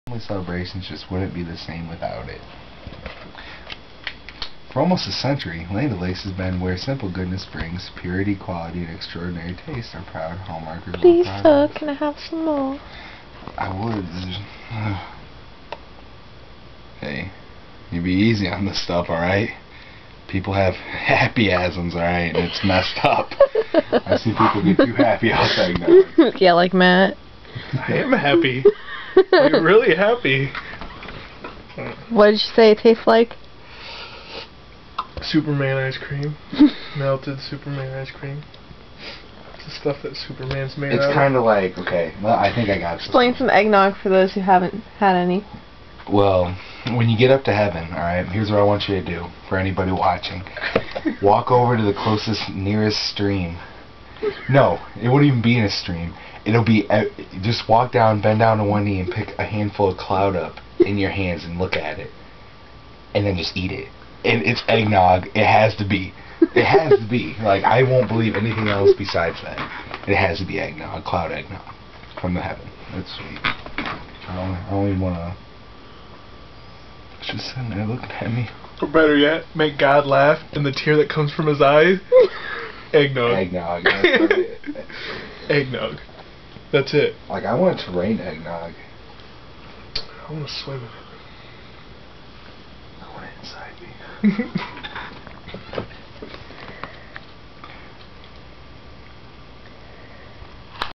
Eggnog Explained
Matt drinks eggnog for the first time ever. Discussion is then had about how amazing it truly is.